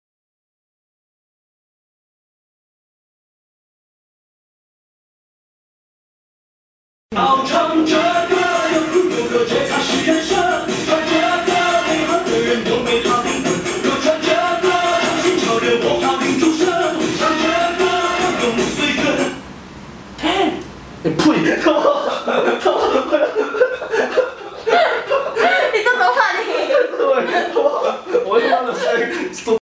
NG BEHIND THE SCREEN OF I wanna be singapore Comedian too! LOL.avi
Lol. my friend who videos for me find this funny lols. should be using 郭富城 Voice but end up using 曾志伟 voice